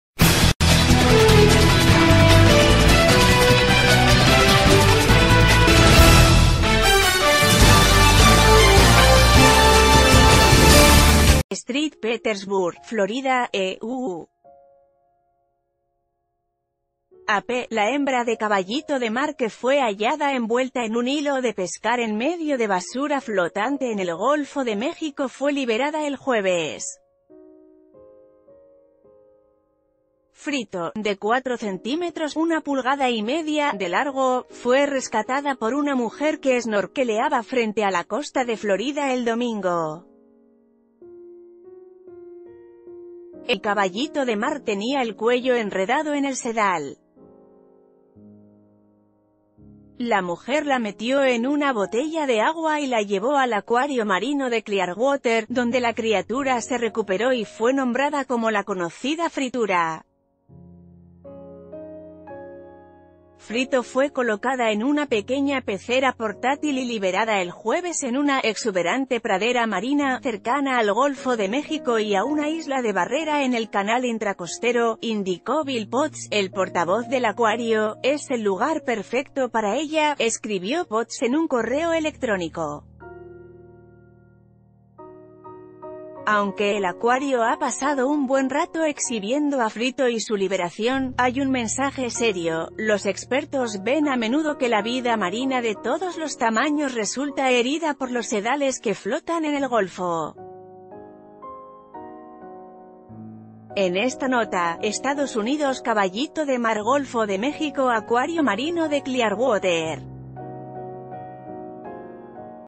Hembra de caballito de mar rescatada es liberada en Florida
ST. PETERSBURG, Florida, EE.UU. (AP) — La hembra de caballito de mar que fue hallada envuelta en un hilo de pescar en medio de basura flotante en el Golfo de México fue liberada el jueves. Frito, de cuatro centímetros (una pulgada y media) de largo, fue rescatada por una mujer que esnorqueleaba fren...